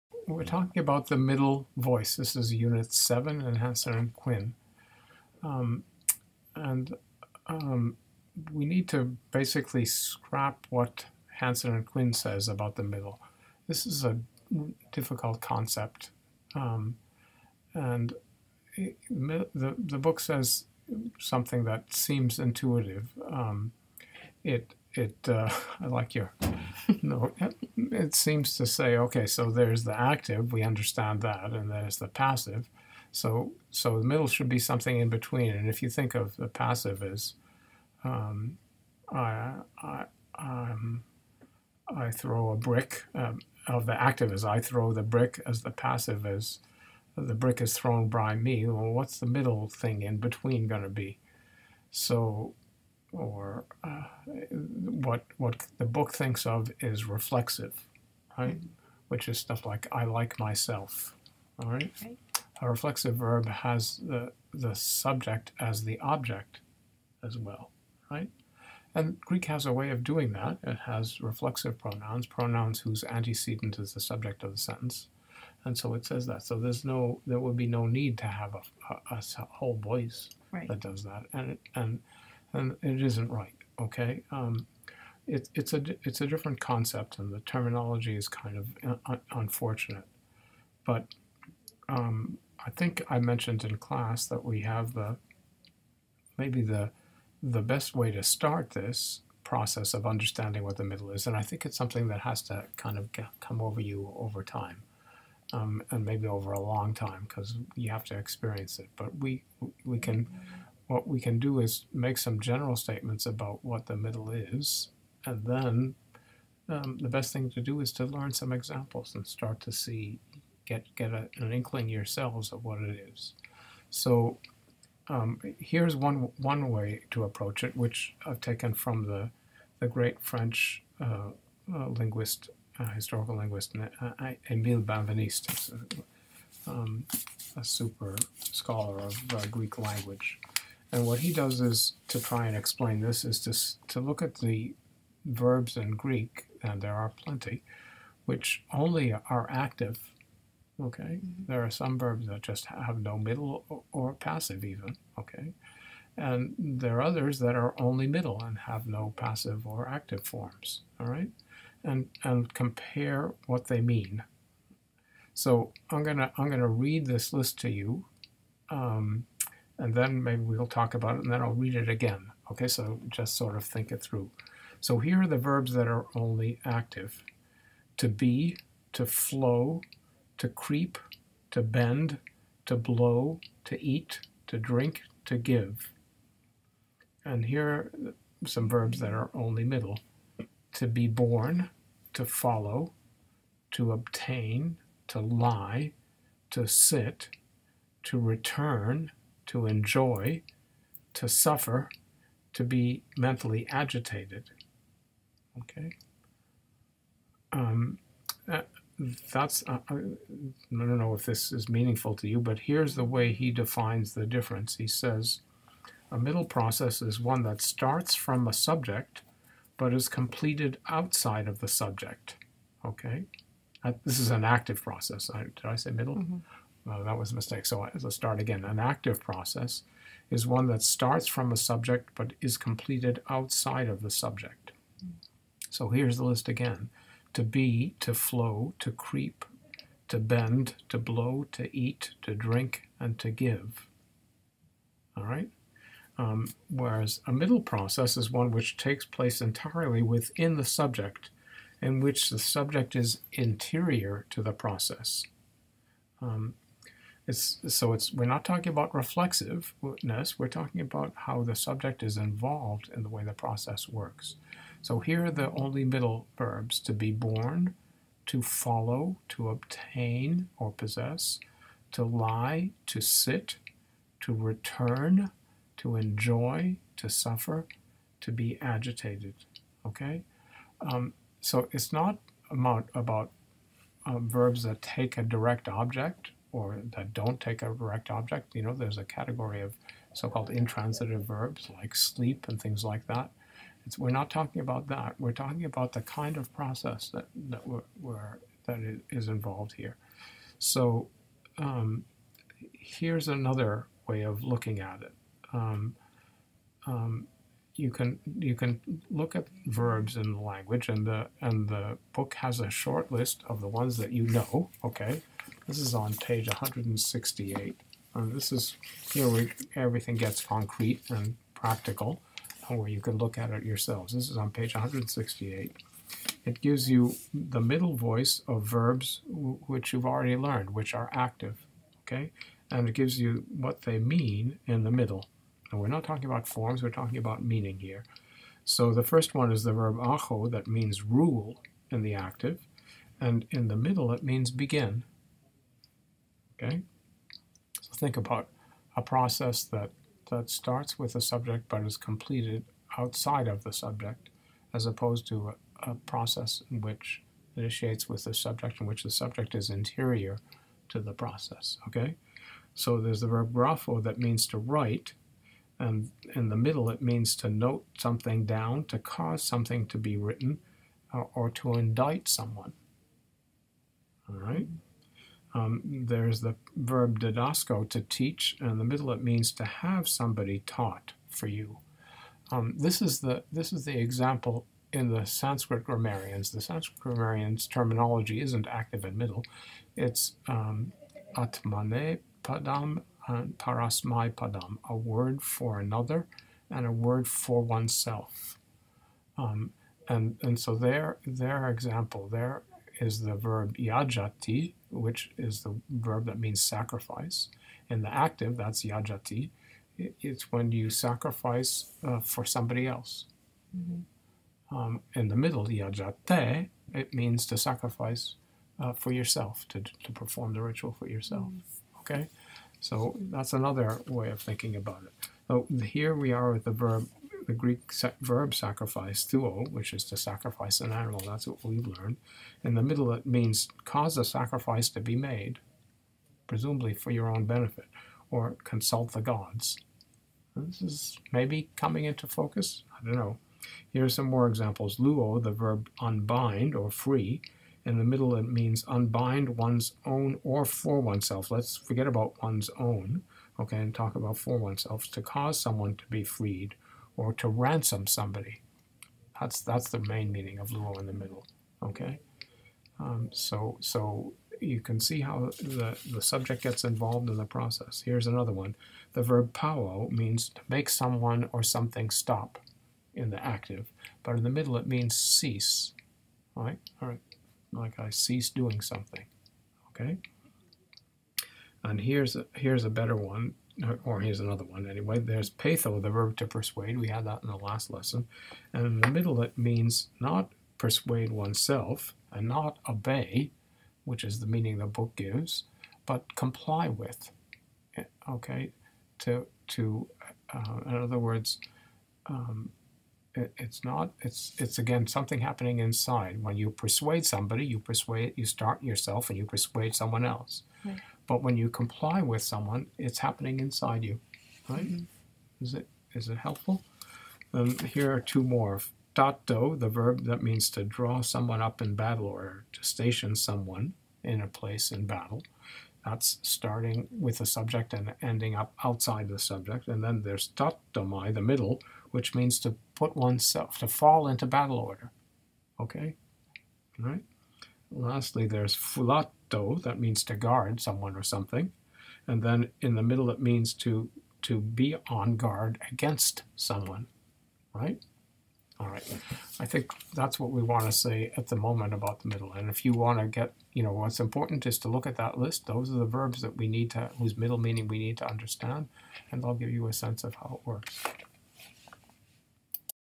Learn Ancient Greek: 36_Unit 7 Meaning of Middle Voice
Leonard Muellner (Professor Emeritus of Classical Studies at Brandeis University) and Belisi Gillespie present all the content covered in two semesters of a college-level Introduction to Ancient Greek course.

Unit 7 deals primarily with the Middle Voice, but Professor Muellner tries to explain the use and function of the Middle in a better way than the textbook does. This video makes some general statements about what "the Middle" is, but it will probably take time and experience for its meaning to sink in.